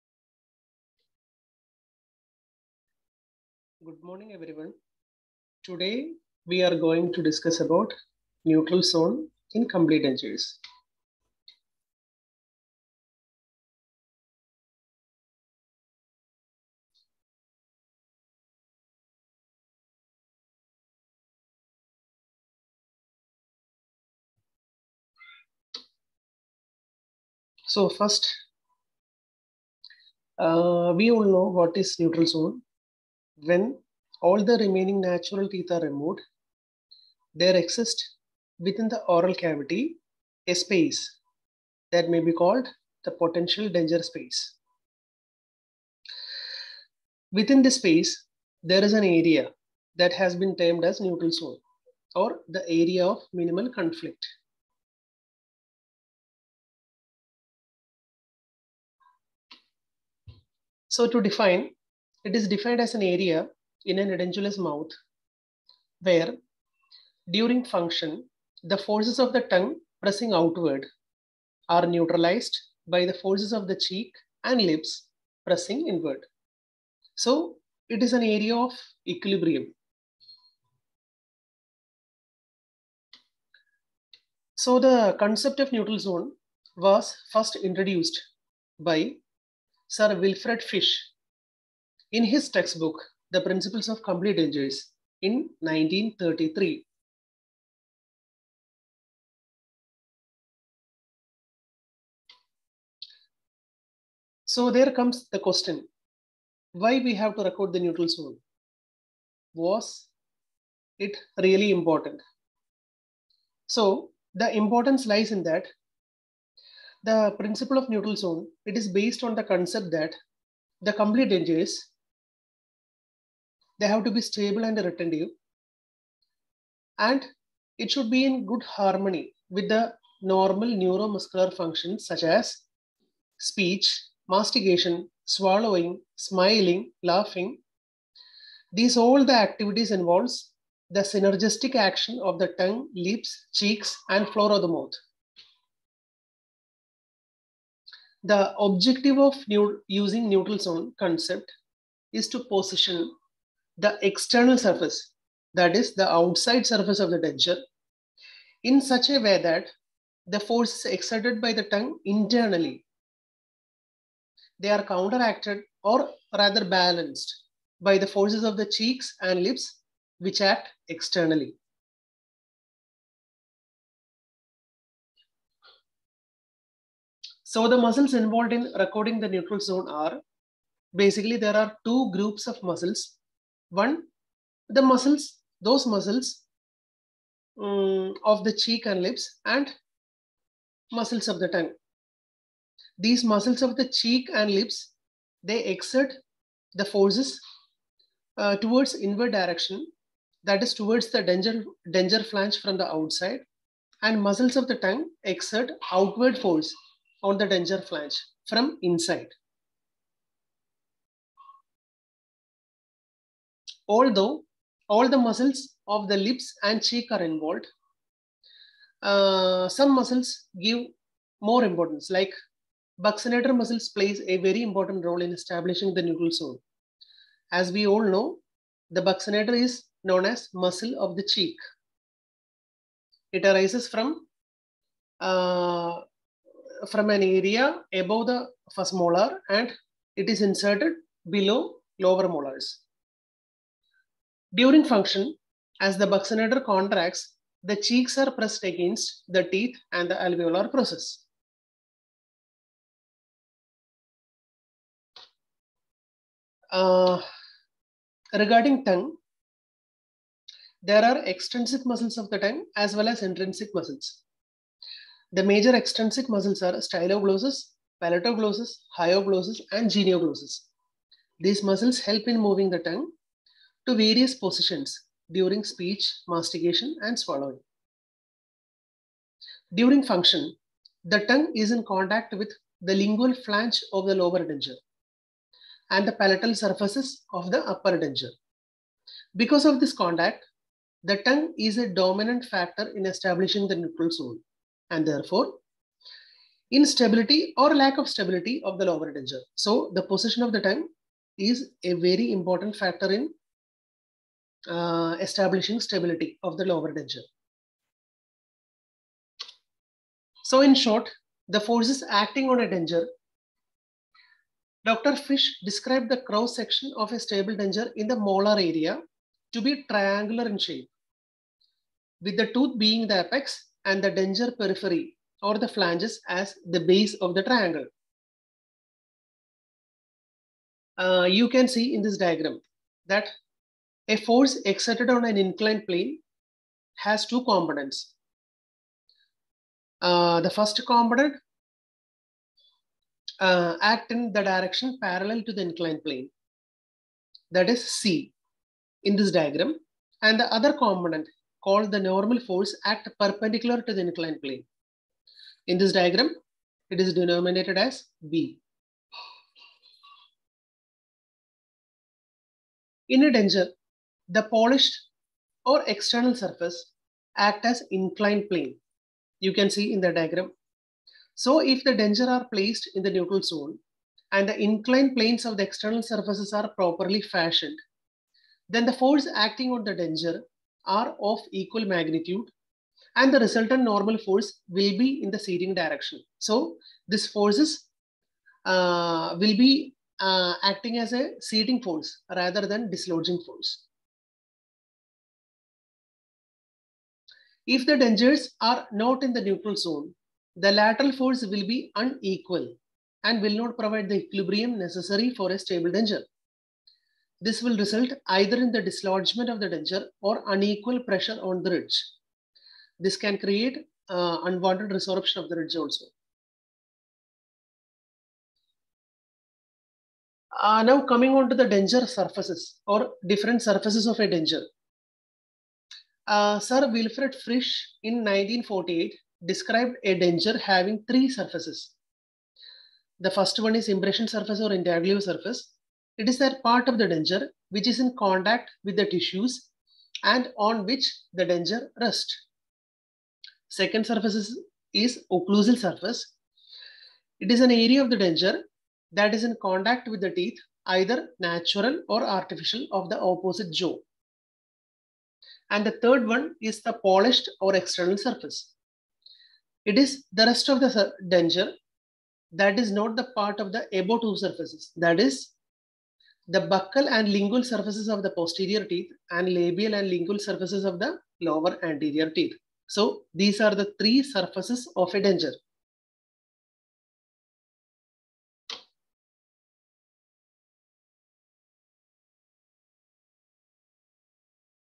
Neutral Zone in Complete Dentures Part 1|Dr Nanda Kumar|Professor|Prosthodontics|MESDC
Topic: Neutral Zone in Complete Dentures Part 1
Faculty: Dr Nanda Kumar, Professor
Department: Prosthodontics
MES Dental College & Hospital
Perinthalmanna
Kerala
India